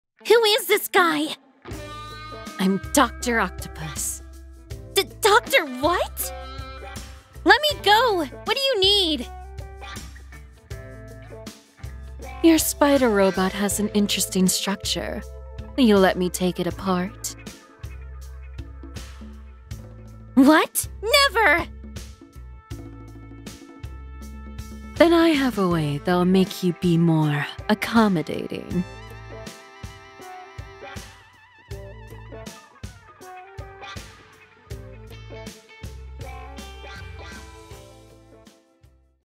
Peni Under Control | Comic DUB
Penny Parker from Spider Man across the spider verse is trapped by DocOc!

If you like my video please consider a subscription❤️!

Voice-over made by DUB
Editing/animation made by DUB